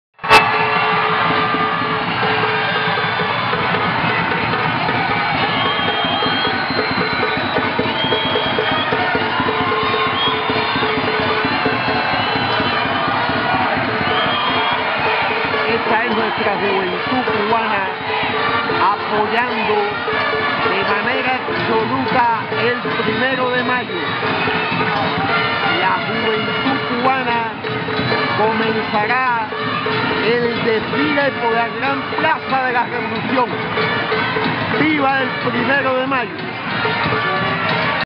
Acto preparativos Primero de Mayo 2017
Acto Provincial de la CTC con vista a los preparativos para el desfile del Primero de Mayo 2017 en la Plaza de la Revolución.